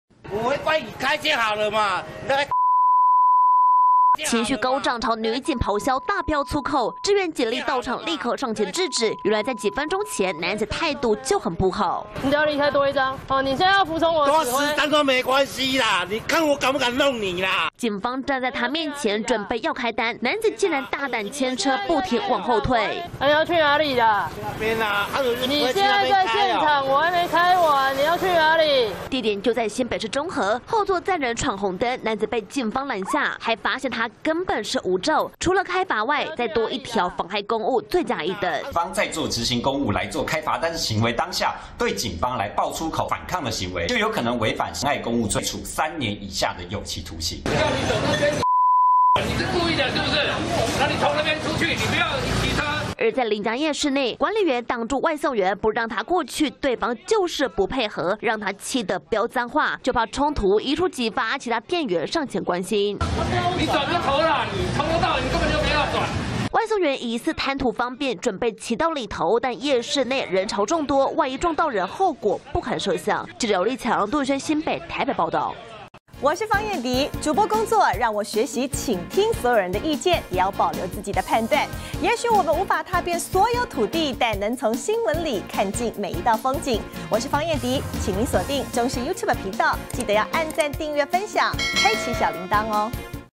20240816中視新聞騎士闖紅燈囂張嗆警 外送 騎進夜市 遭攔爆衝突│中視新聞 20240816
『立勤小學堂』
本來被開開罰單就只是繳罰鍰，沒想到辱罵警方變成刑事案件，妨害公務最多可以處三年以下有期徒刑，還是不要太歲頭上動土。
新聞網址：
『中視新聞』騎士闖紅燈囂張嗆警 外送"騎進夜市"遭攔爆衝突
違規在先，還大膽罵警察!新北市有名騎士，在員警面前闖紅燈，上前開單時，不但不配合，還大飆粗口，甚至嗆"開10張也沒關係"，結果被警方再揪出，他竟然還沒有駕照!